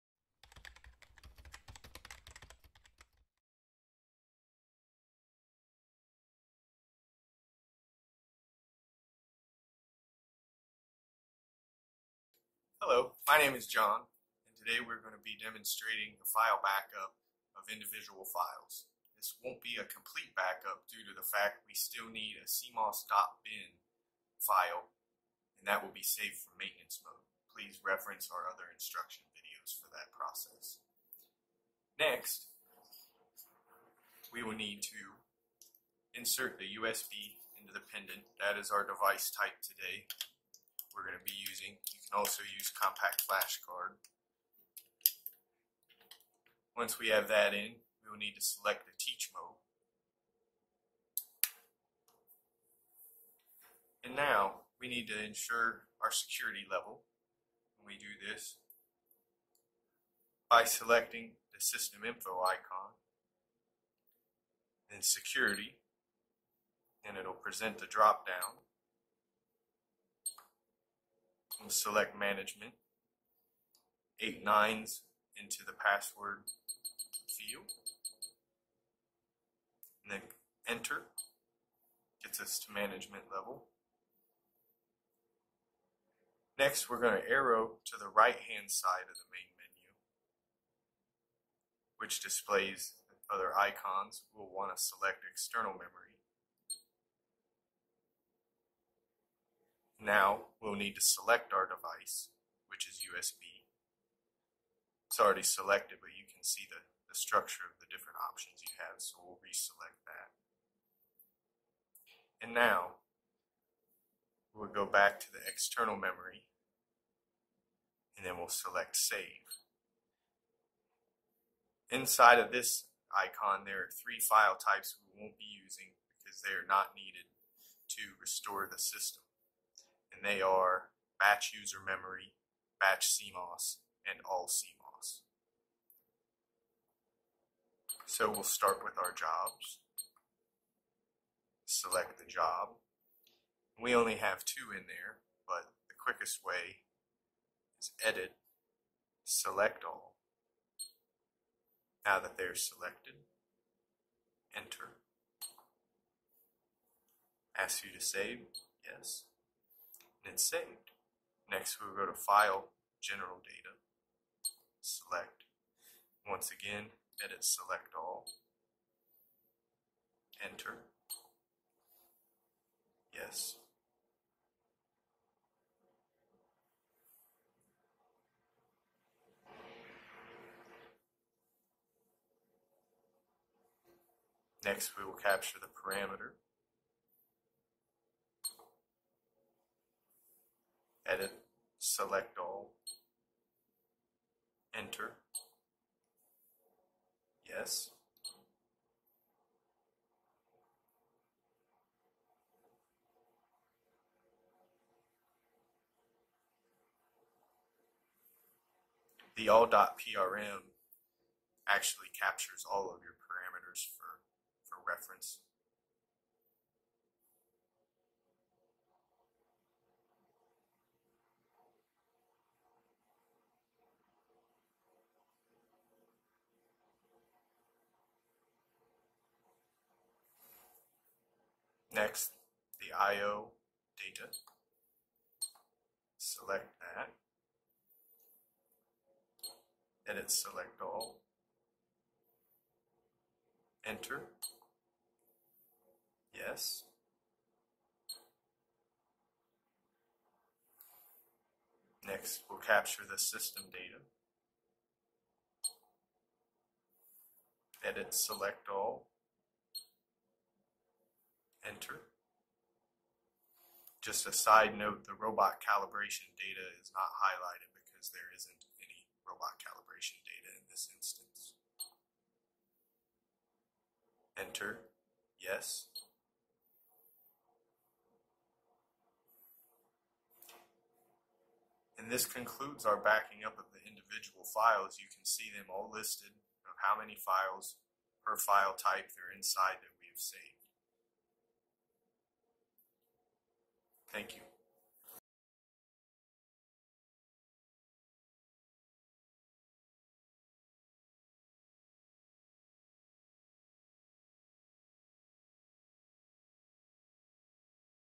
DX100 - Individual File Backup
How to perform an individual file backup on the Yaskawa Motoman DX100 robot controller.